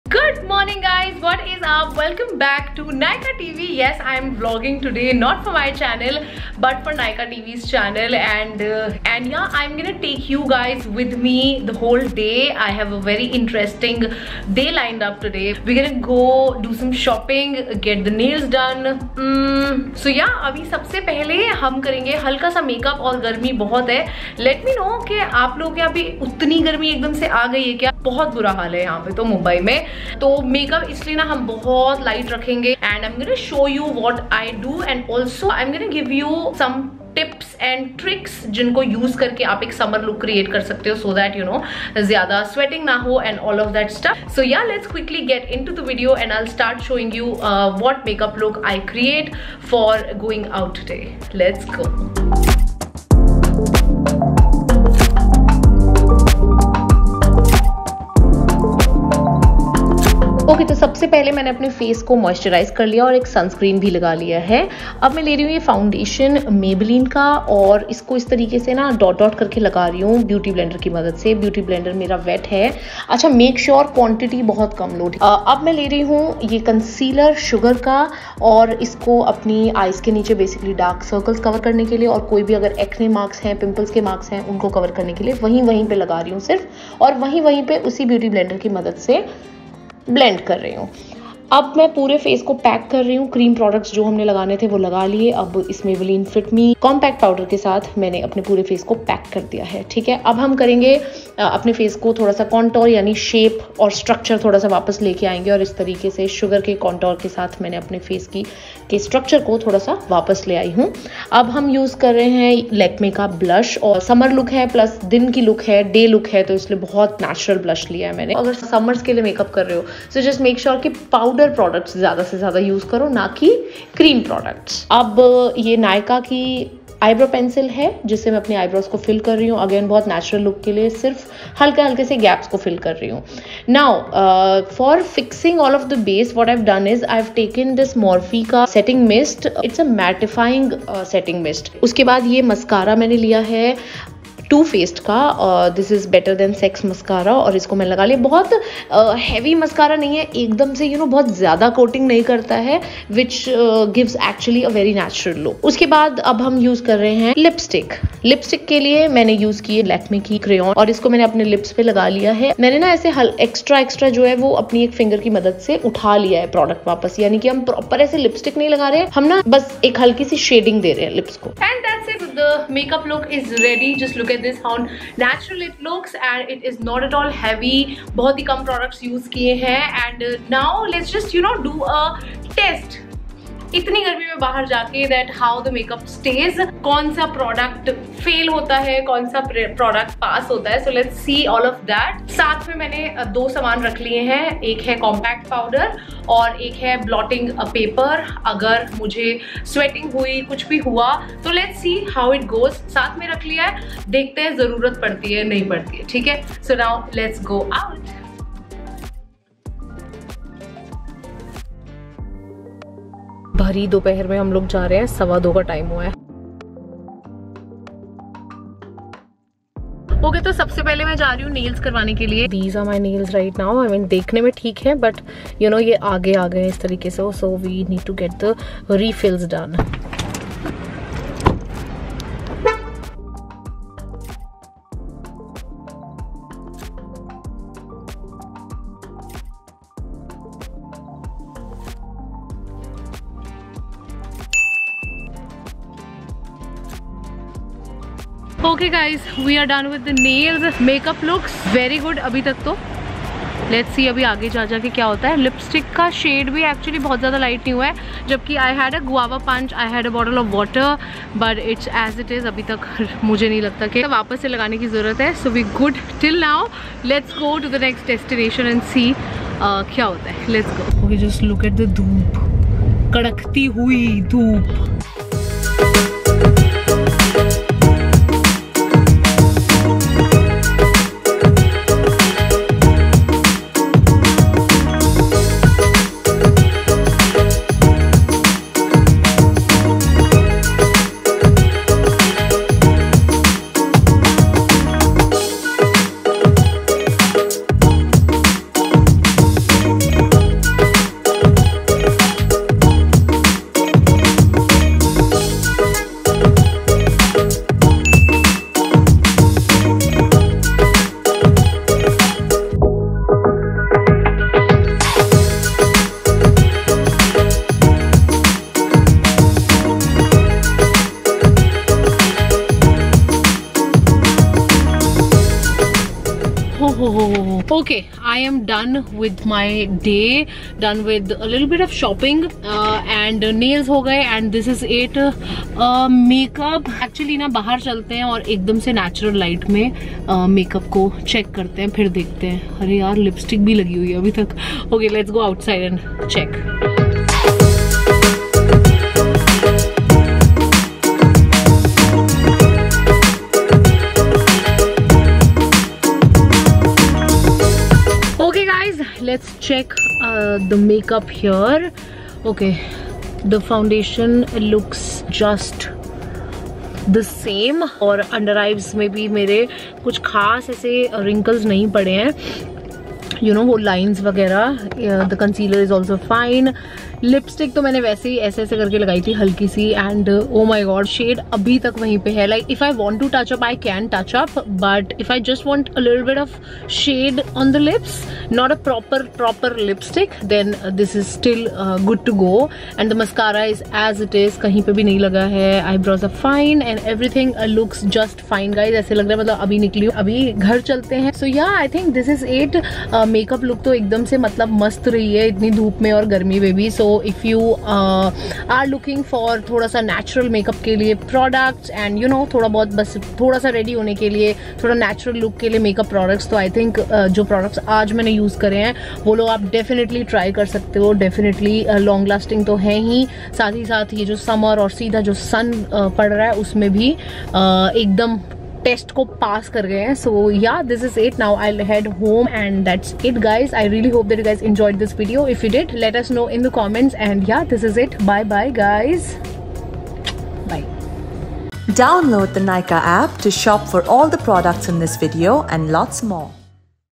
Spend the Day with Me Ft. @sethijyoti | Summer Makeup Tutorial And Testing | Nykaa
Won't it be fun to join your favourite influencer as they go about their day?
If your answer is yes, then Nykaa has a surprise for you.
Jyotii Sethi will be vlogging around, showing you what a day in her life looks like. From spilling the beans and testing her summer makeup look in this deaded heat to a shopping spree!
So, tag along and catch up on all the fun things Jyotii has done as she captures all these moments for you in this Nykaa TV vlog!

SUGAR COSMETICS Contour De Force Mini Bronzer in 02 Woody Wonder

Liked our videos? Check these out!

How To Get Your Makeup To Last All Day Ft. Jovita George | Longlasting Summer Makeup | Nykaa